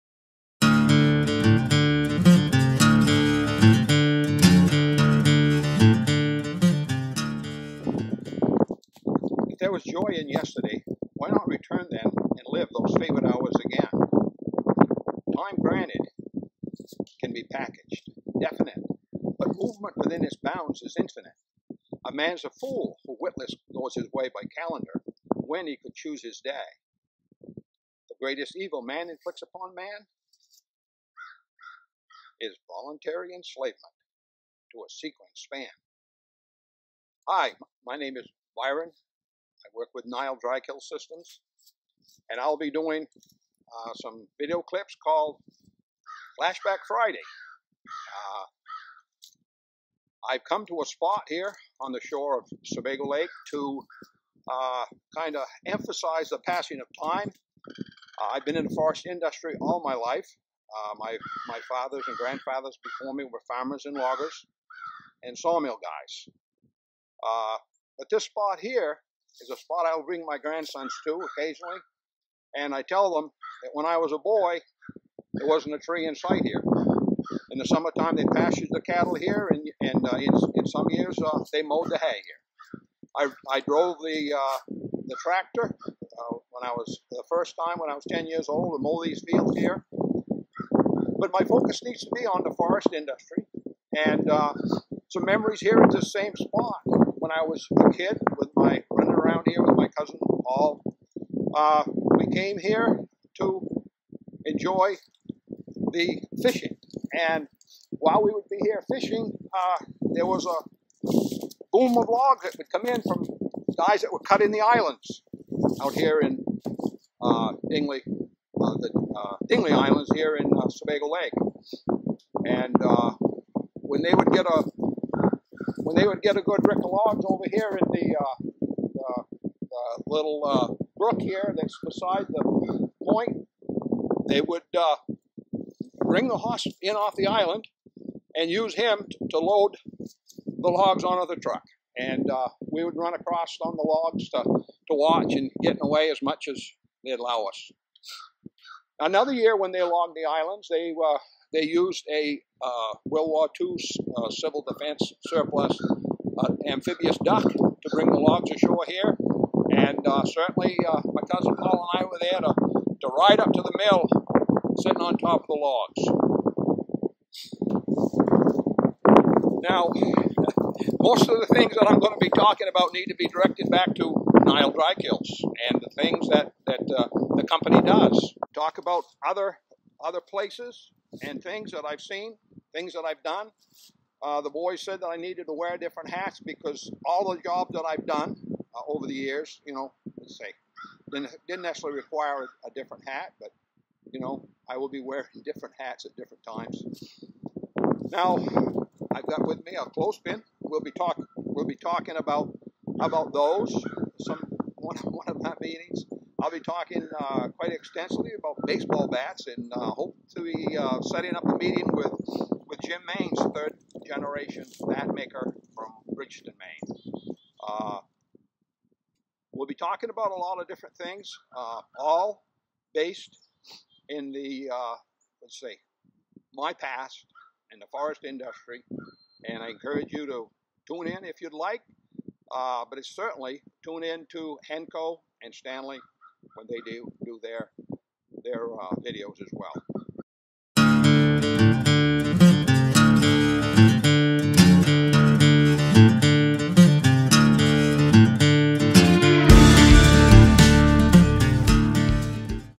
Flashback Friday Episode 1
Here it is guys Our very first "Flashback Friday with Byron Chute" episode but because of some auto-posting difficulty you're getting it on this beautiful Saturday!

Pop open an ice cold beverage of your choosing and learn a little bit about the "Good Ol' Days" in the forest industry.